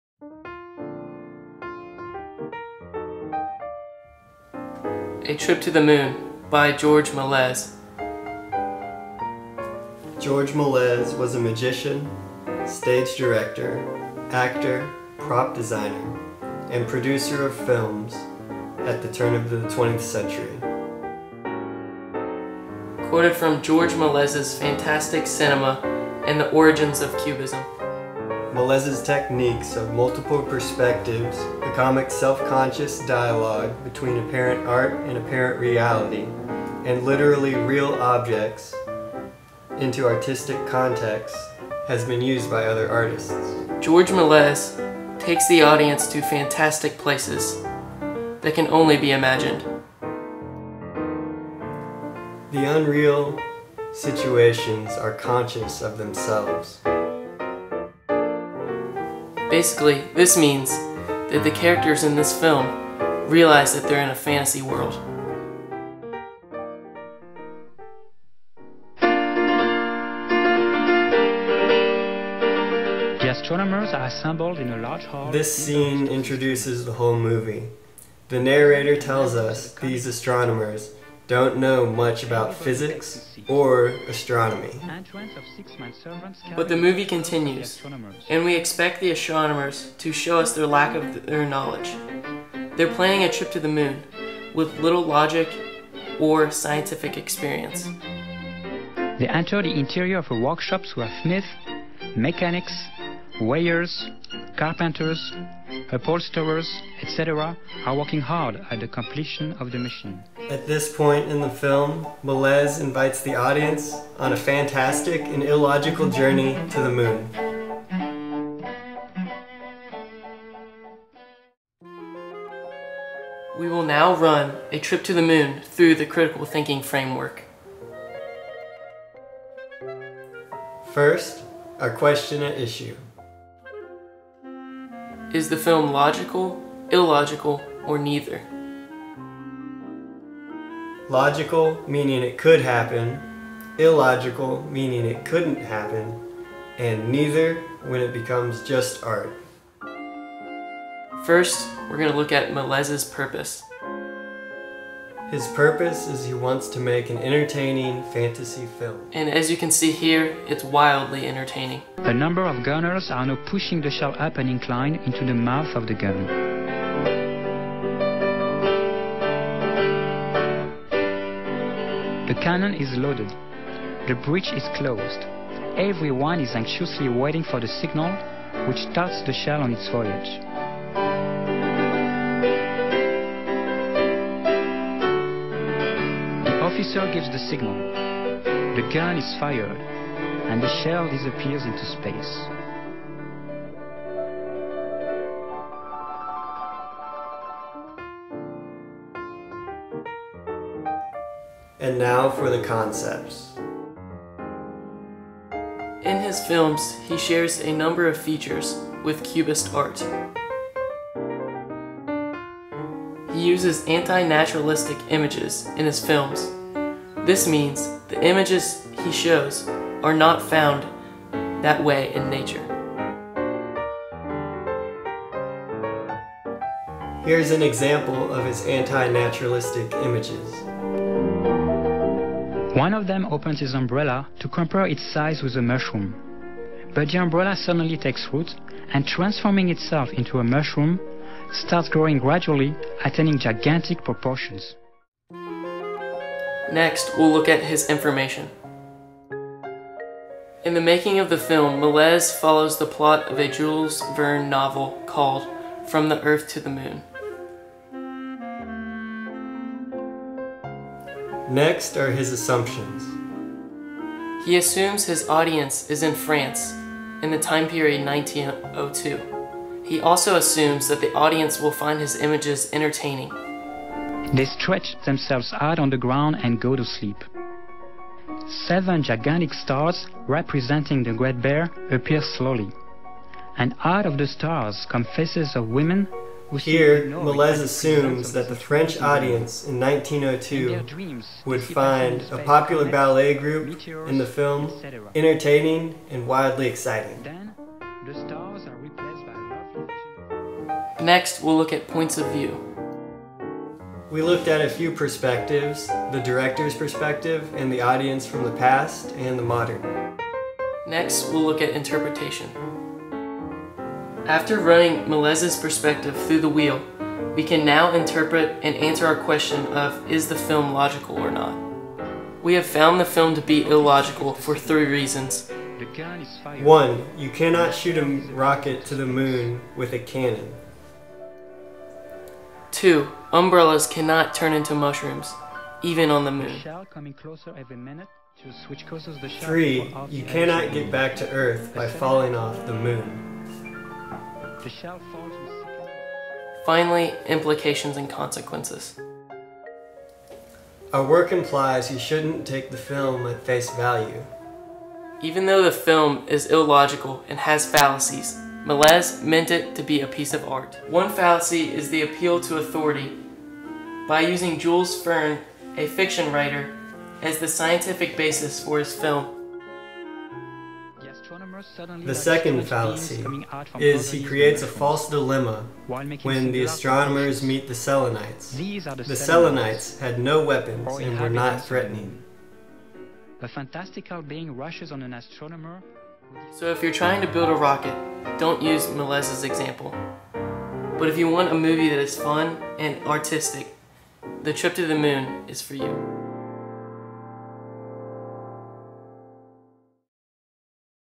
Thinking Critically about "A Trip To The Moon"
This is a student film from a Critical Thinking (PHIL 211) class that applies Paulian Critical Thinking Theory to Georges Melies' silent film, "A Trip to the Moon" from 1902. Student have granted permission for the film to be distributed for educational purposes, and it was screened at the "Lights, Camera, Ideas to Action: Real to Reel" critical thinking film festival at The University of Louisville in 2011.